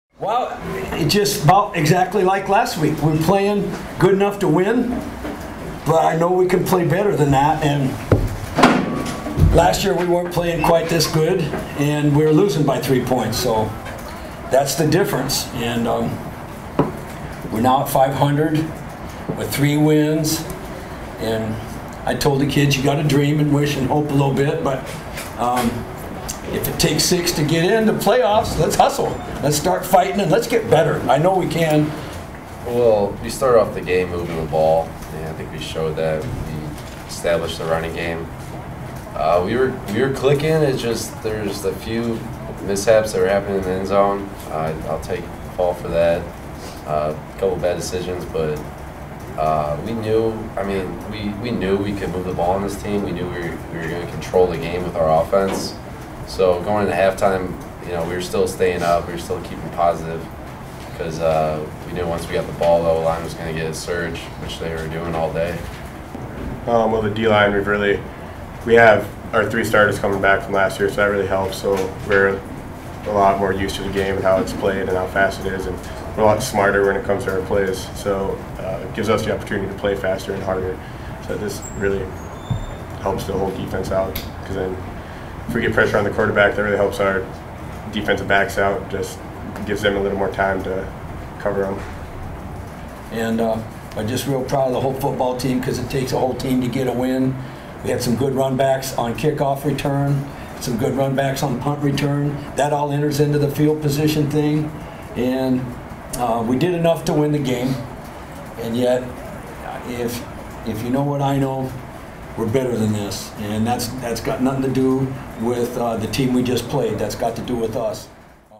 Indiana State - USD reaction
Head coach Joe Glenn, quarterback Kevin Earl and defensive end Drew Iddings share their thoughts after USD's 17-14 win over Indiana State.